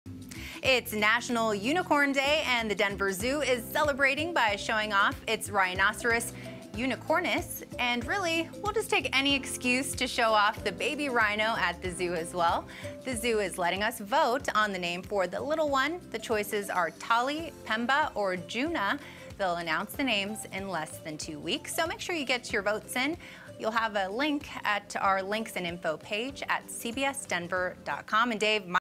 Denver Zoo Celebrates 'National Unicorn Day' With One-Horned Rhino
Unfortunately, there are no actual unicorns at the Denver Zoo but zookeepers still found a way to celebrate National Unicorn Day.